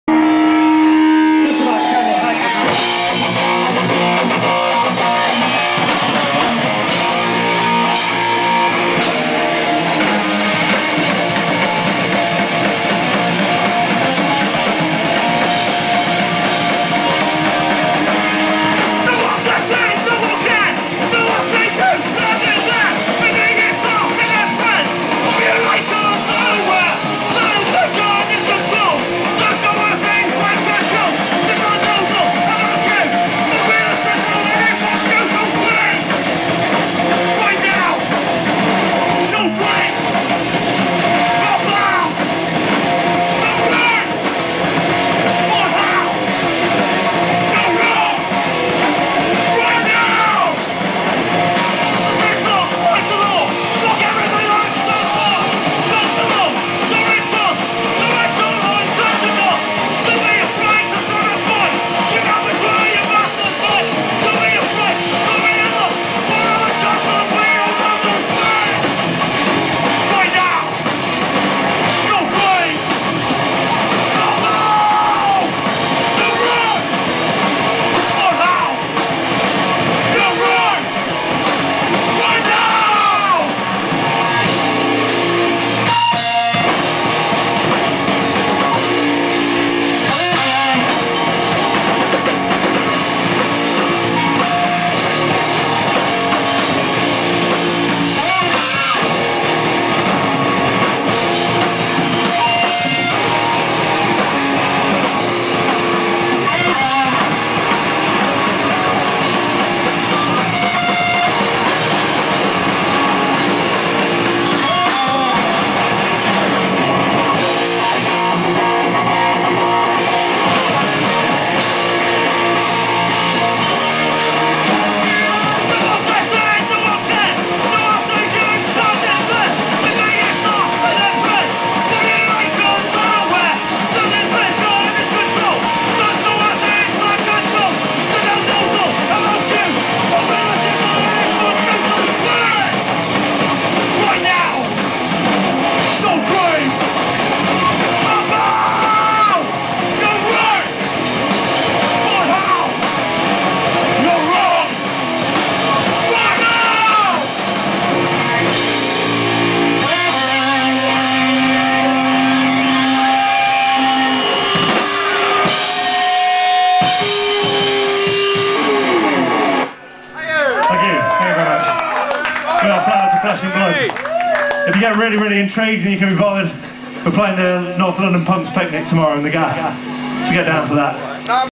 Scream Gros Jan 10.MPG
Screaming down the Grosvenor for our mates at Reknaw, Jan 10, in support of Police Bastard